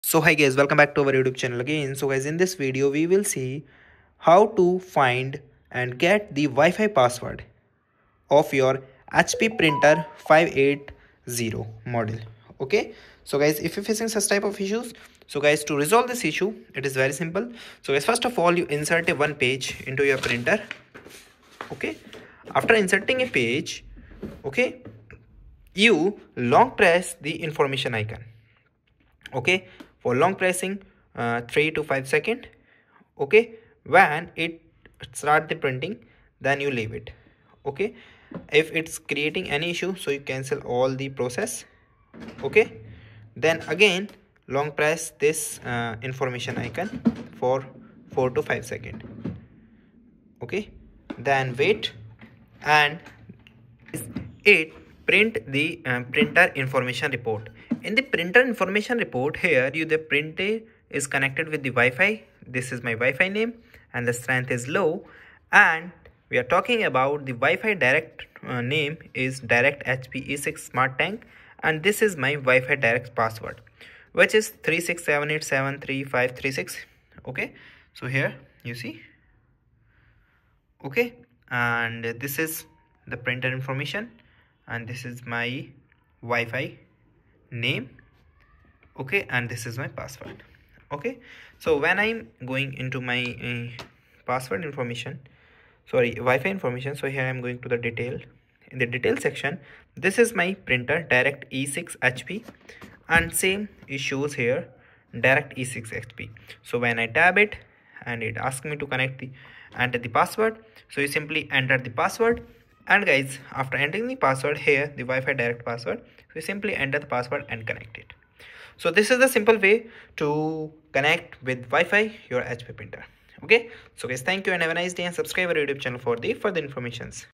HP Smart Tank 580 Printer: How To Find/ Get WiFi password - Know WiFi Password
In this informative video, we will guide you through the process of locating and retrieving the WiFi password for the HP Smart Tank 580 Printer. Whether you are setting up your printer for the first time or troubleshooting connectivity issues, this step-by-step tutorial will provide you with the necessary instructions to access your WiFi credentials effortlessly.